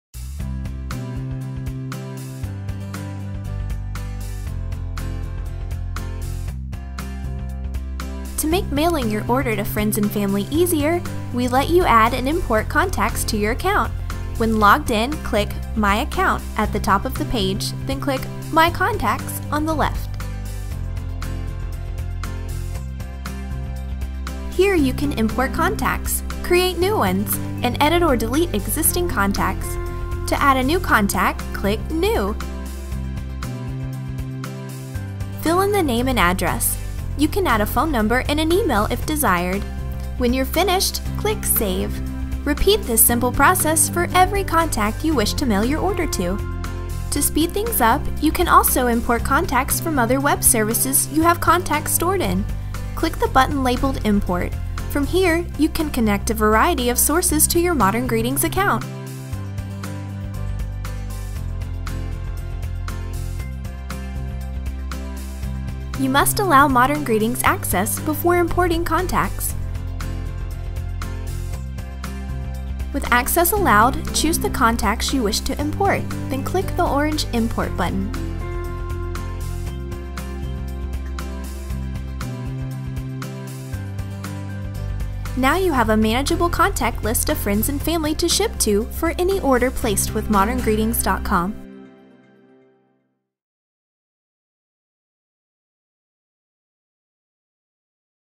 Adding Contacts - Tutorial from ModernGreetings.com
- Mail you cards directly to your friends and family on ModernGreetings.com. Watch to see how you can add contacts individually or connect other online service to pull contacts from there.